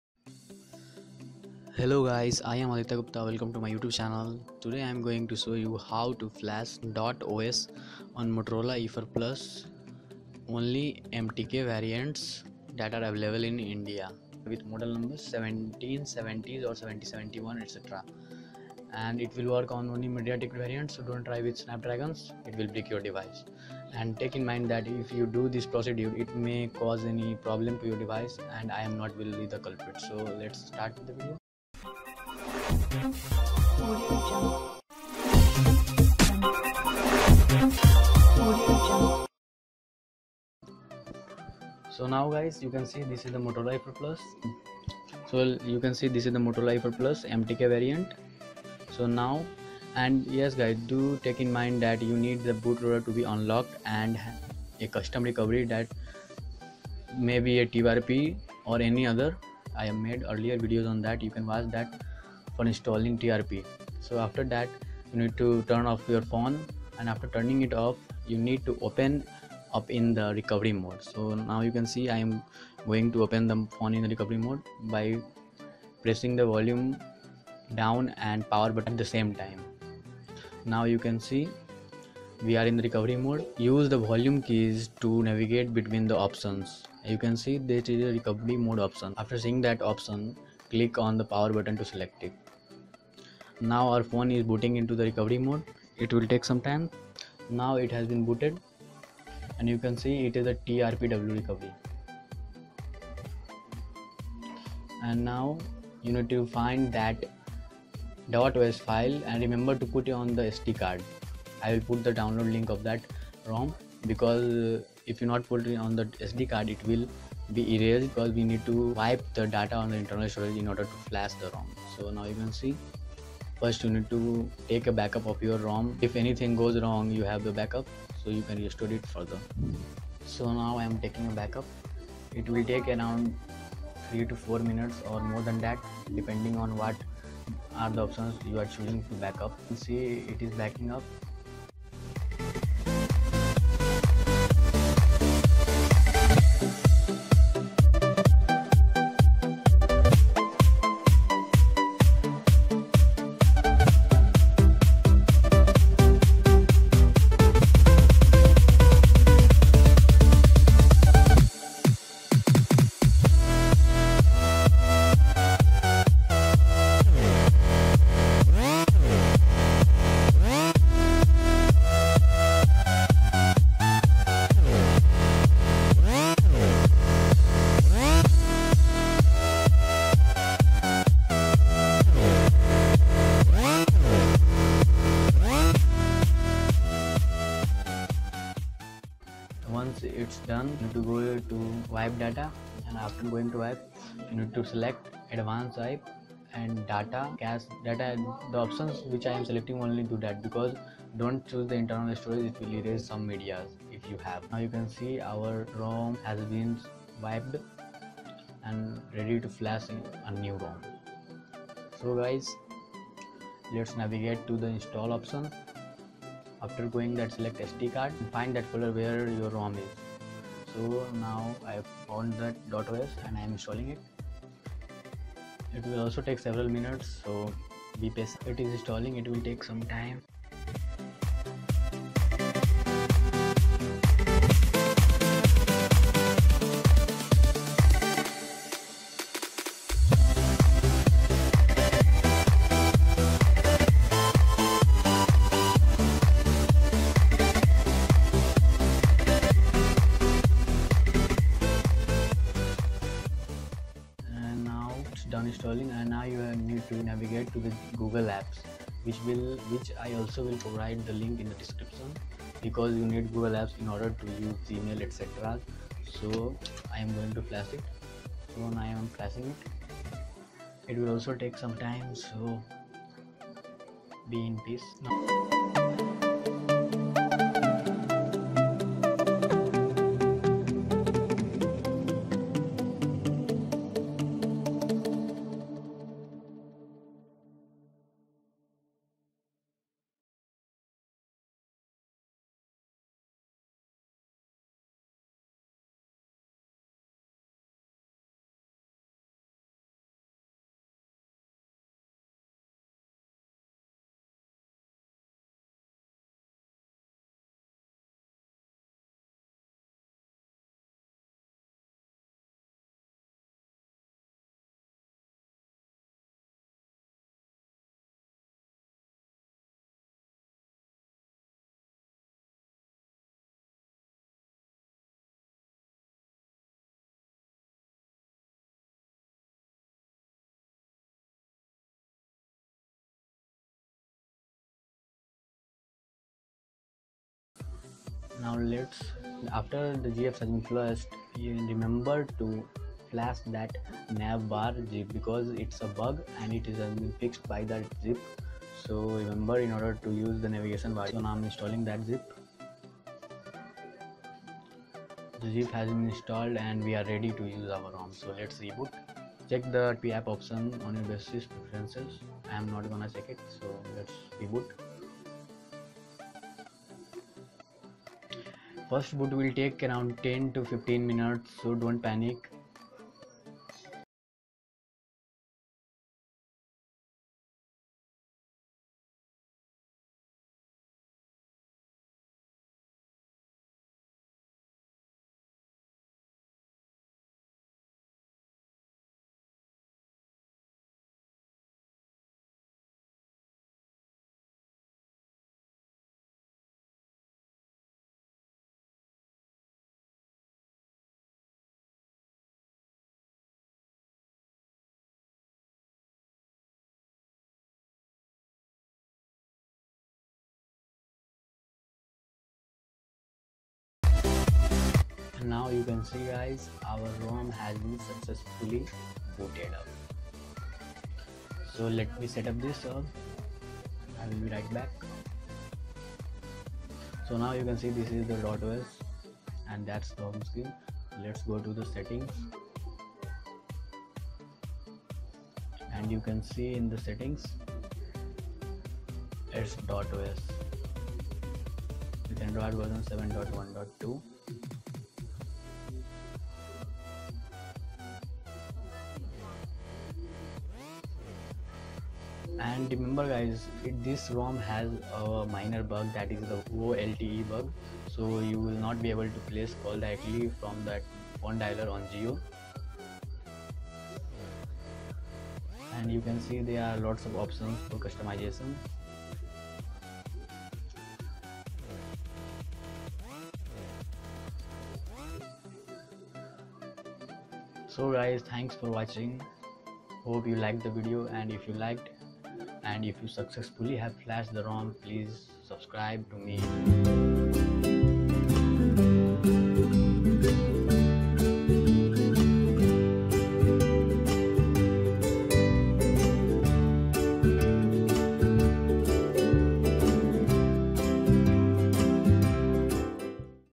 How to flash Dot OS on Moto E4 plus xt1770 MEDIATEK
How to flash Dot os on moto e4 plus.

Both downloads are in the above link and download 2nd build.

  LET'S BE A BIT OPEN 😊😊